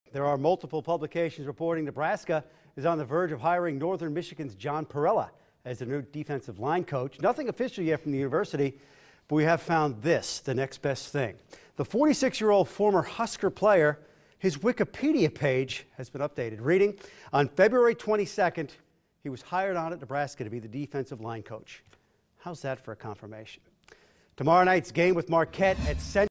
Former husker John Parella hired as defensive line coach
Nebraska head coach Mike Riley announced two new additions, the hiring of former Huskers John Parrell as the defensive line coach and Billy Devaney as executive director of player personnel.